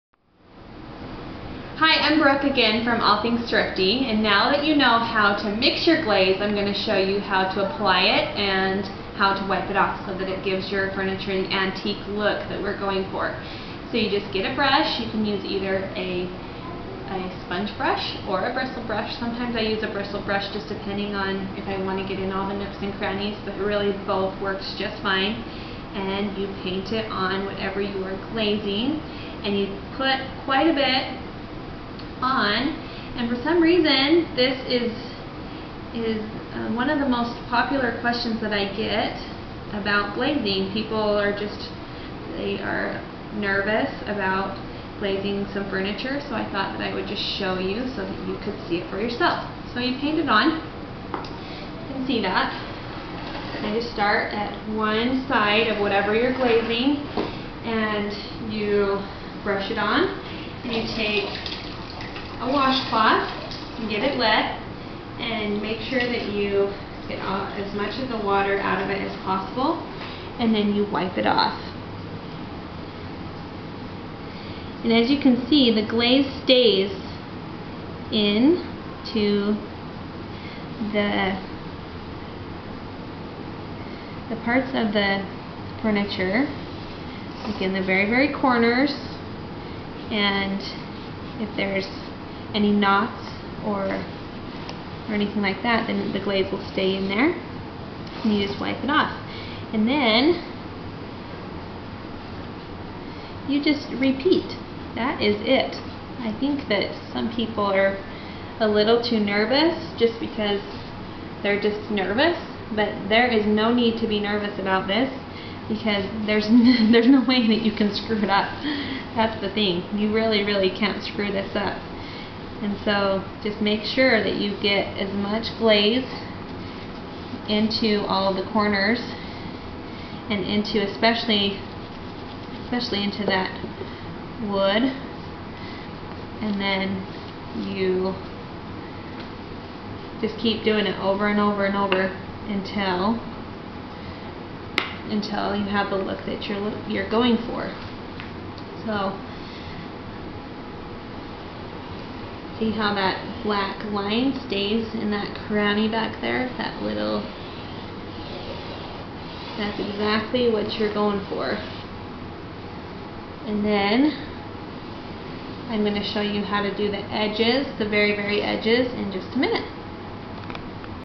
How to glaze furniture
This video will demonstrate how to glaze furniture to create an antique finish.
Here are some links to answer all of the questions: Glazing 101
and Glazing Furniture FAQs: